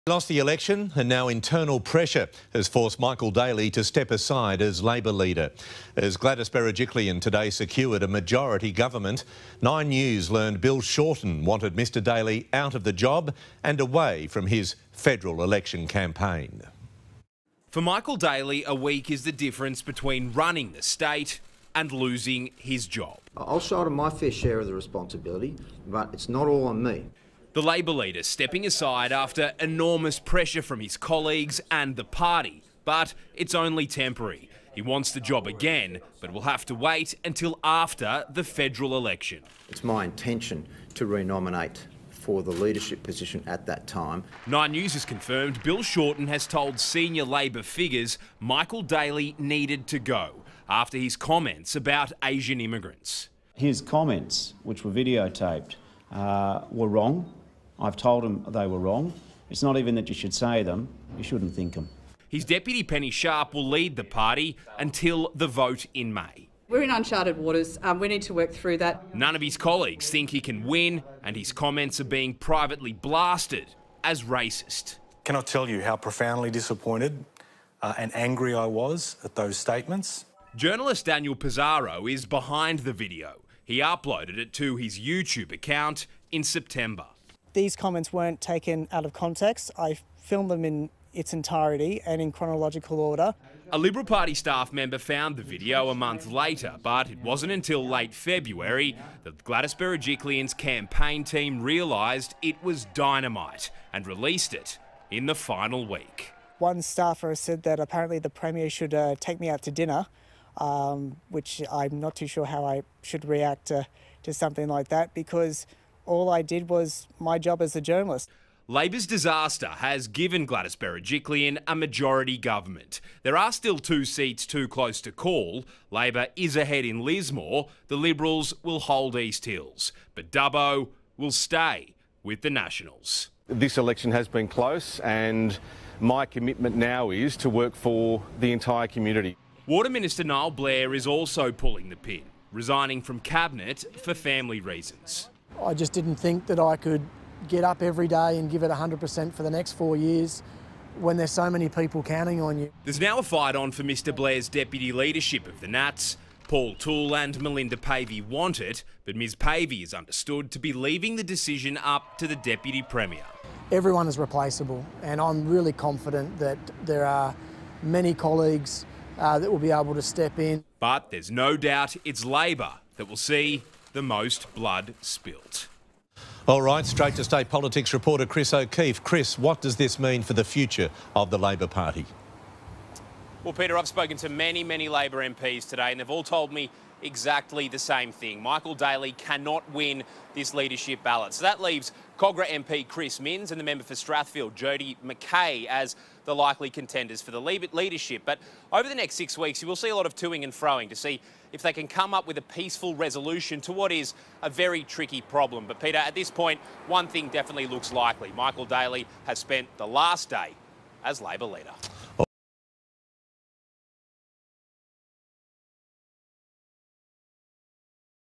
NSW Labor leader Michael Daley to step down after losing state election | Nine News Australia
Following a shocking final week of the campaign, Michael Daley is to stand down as the NSW Labor leader.